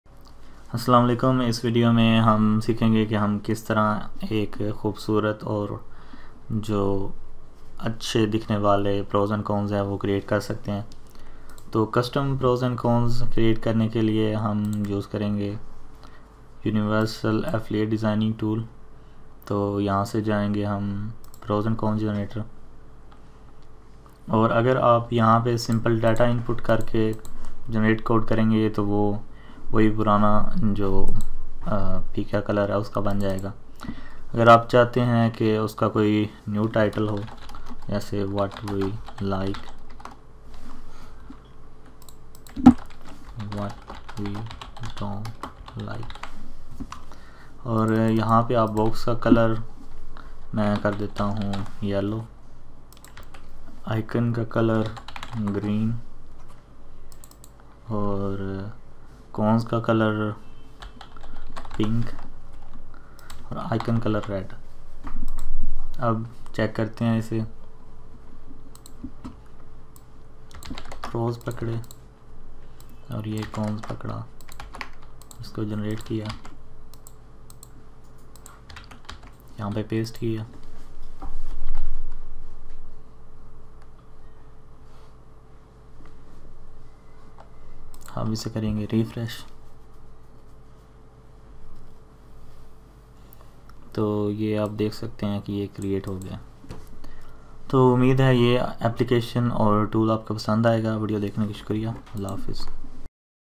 How to Design Custom and Beautiful Pros and Cons using Universal Designer
💥💥Vieo Chapters💥💥
00:00 Universal SEO Tool
00:48 Building Dofollow Comment Backlink
03:50 Building Dofollow Forum Backlinks
14:32 Ending

This video includes: